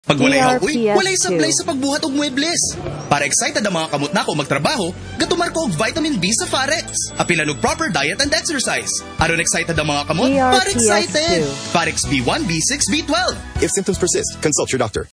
Pharex Vitamin B1 + B6 + B12 Radio Ad 2024 15s (Cebuano Version)
This is the 2024 Radio Ad for Pharex Vitamin B1 + B6 + B12, Pharexcited! (Cebuano Version)

DRPS Two 7055: Pharex Vitamin B1 + B6 + B12 Radio Ad 2024 15s (Cebuano Version)